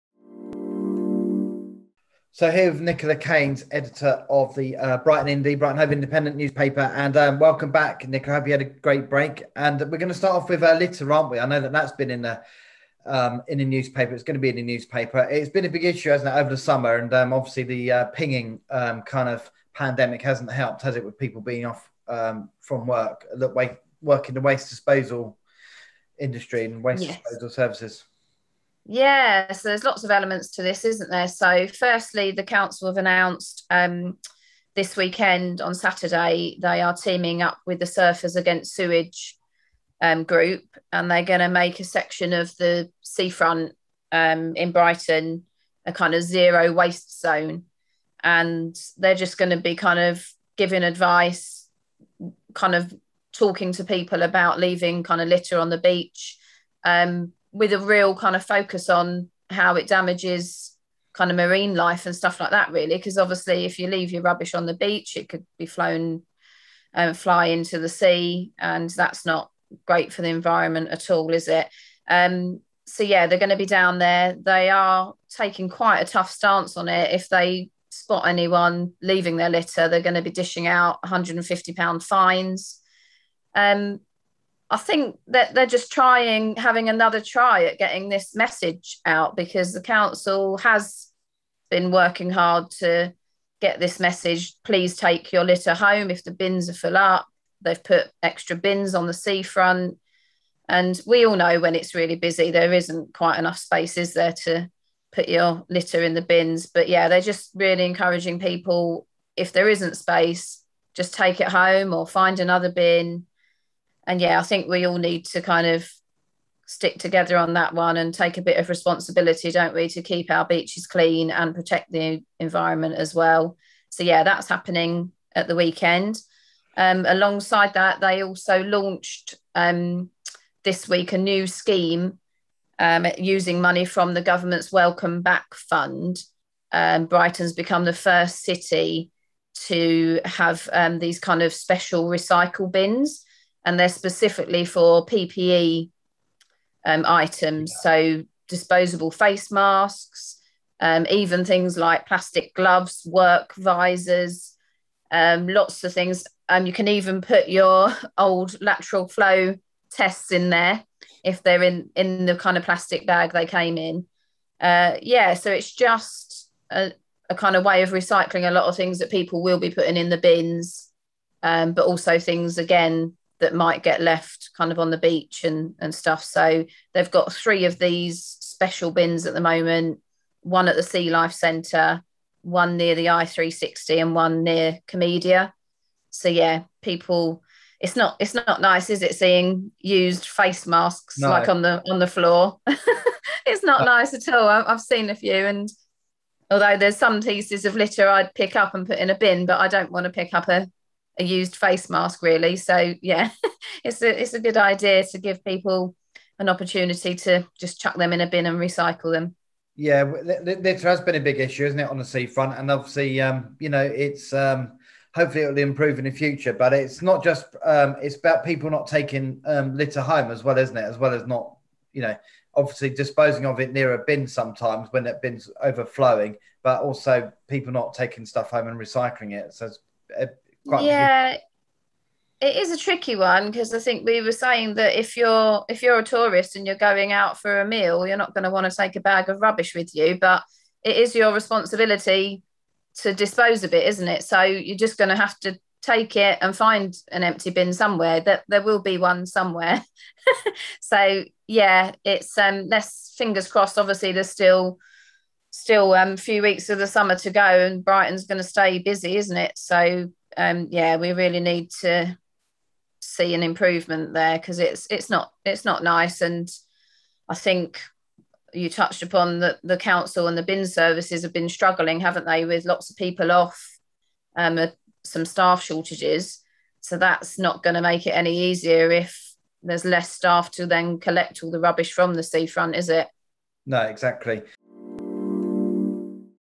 Nicola Caines talks solutions to the Brighton Seafront Litter Problem
Latest TV News Editor, Mark Walker, spoke with Nicola Caines, Editor of the Brighton and Hove Independent, about the issue of Littering in Brighton and Hove, especially effecting the beach area, and solutions to this problem.